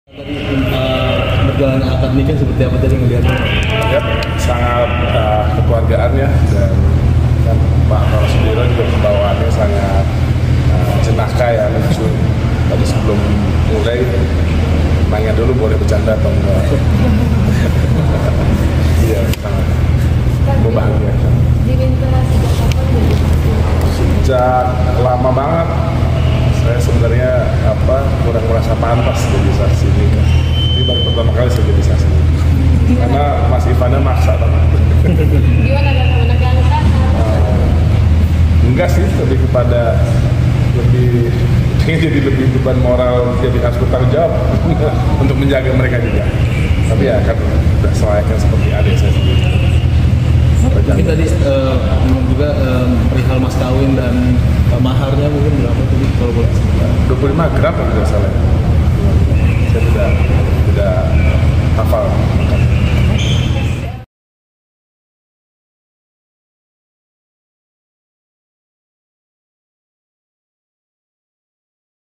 Hadir Jadi Saksi, Menpora Bocorkan Tingkah Kocak Tora Sudiro Sebelum Nikahkan Anak
Putri Tora Sudiro, Azzahra Nabila Sudiro menikah dengan lelaki pujaan hatinya, Muhammad Ivan Lubis pada hari ini, Minggu (25/8/2024). Prosesi akad nikah digelar di kawasan Mega Kuningan, Jakarta.